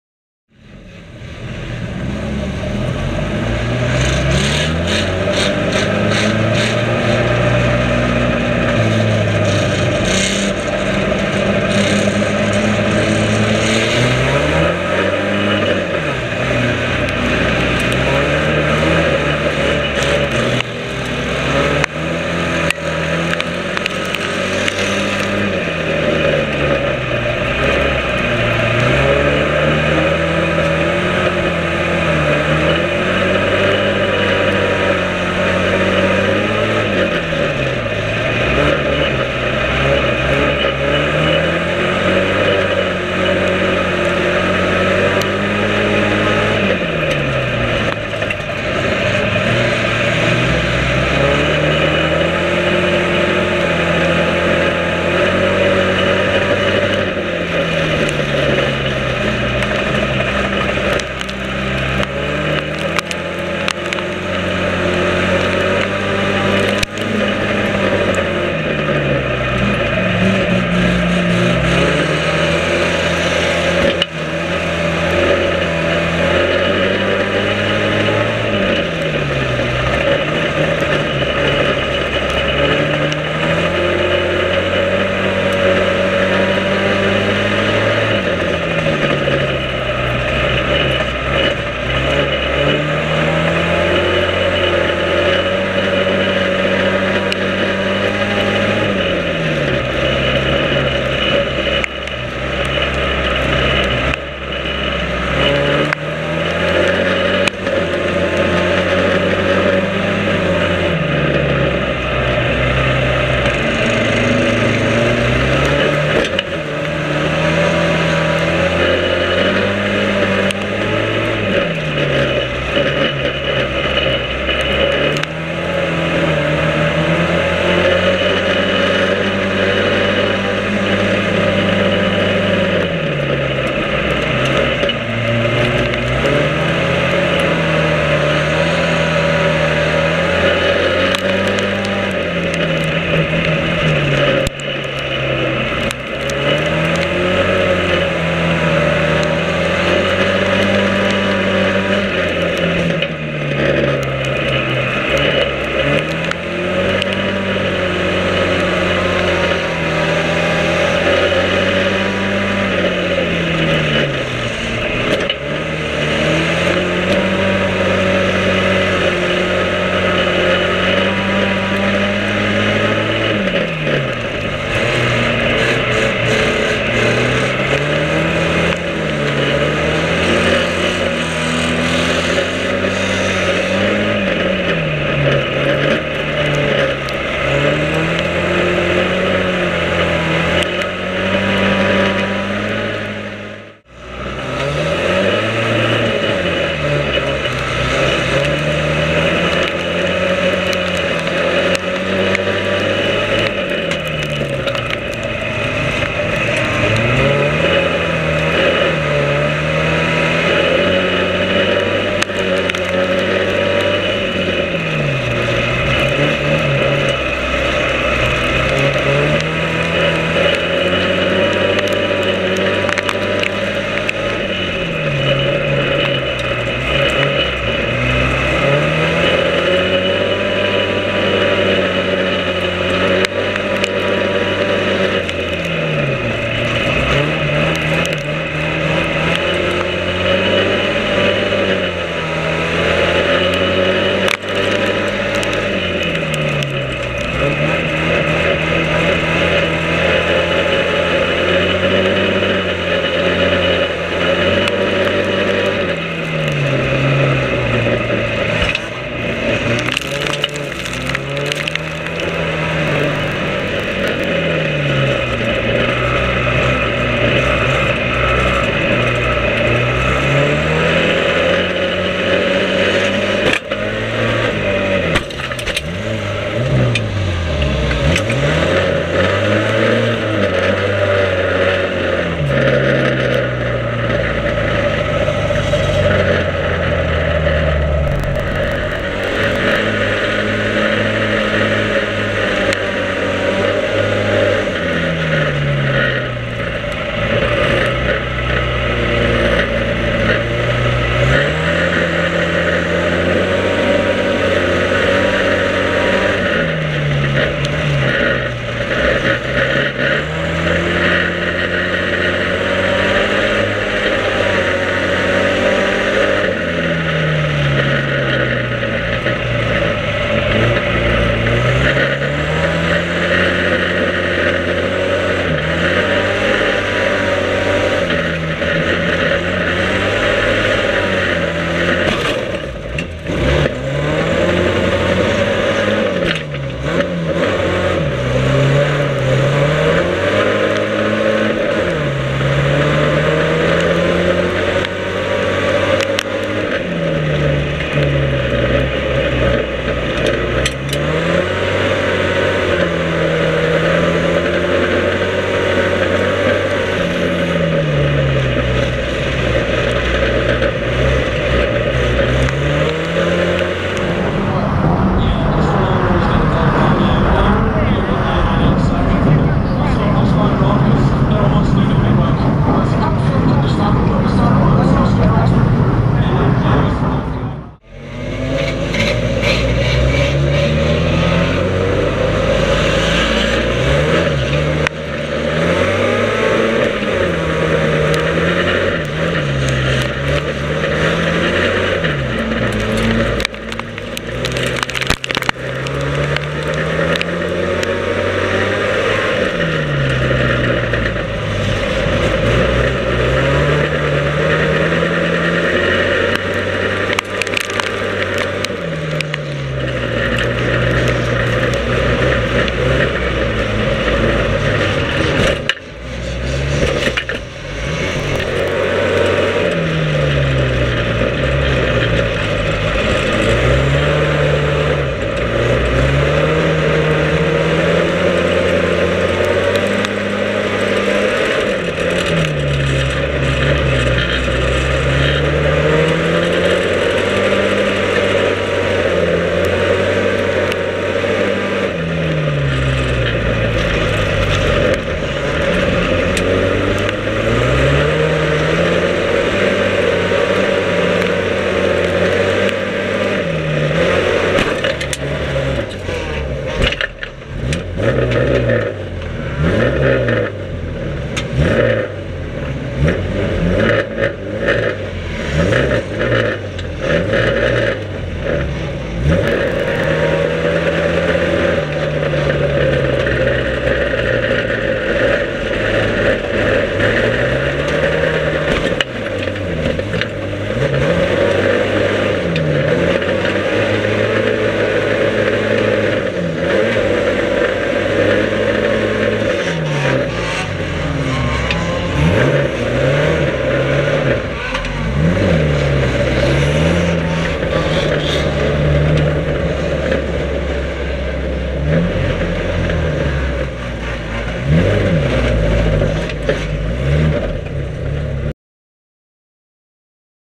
In Car Camera Banger Racing 100 Callum White Icebreaker 2024 Kings Lynn
The on board camera with the Jaguar XJ6 of Callum 'cock' White at the 2024 Kings Lynn Icebreaker. Sit back and enjoy a ride along with Callum, with some clips from the outside edited in as well. Taken from the full DVD production of the event that is on sale